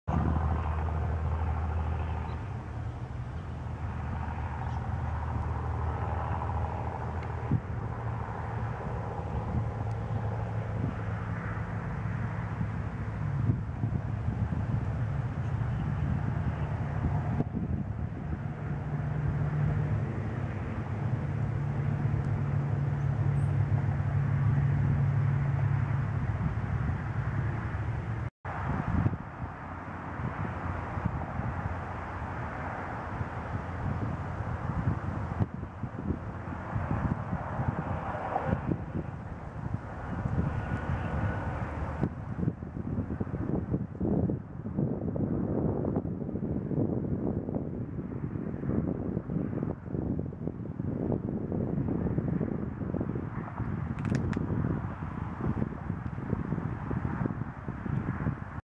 Autumn on Crusher Hill
Panoramic sweep of the vineyards surrounding Crusher Hill in Napa, California.